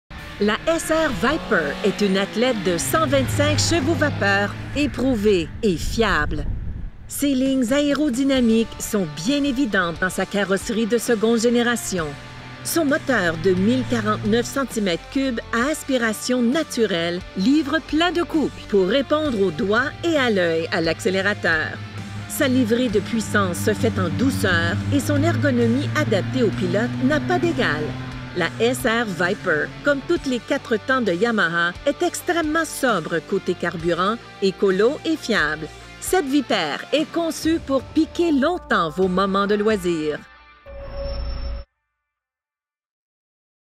La SRViper L-TX GT 2022 Yamaha
La SRViper est une véritable athlète de la classe des 125 HP. Ses lignes aérodynamiques épurées sont reflétées dans sa carrosserie de seconde génération. Son moteur de 1 049 cm³ à aspiration naturelle déploie amplement de couple musclé pour livrer d’excellentes réponses à l’accélérateur. Sa livrée de puissance se fait en douceur et son ergo est de la plus fine pointe. La SRViper, à l’instar de toutes les Yamaha, est extrêmement efficace côtés rendement énergétique, respect de Dame Nature et fiabilité. Elle est conçue pour vous accompagner pendant de très nombreux hivers!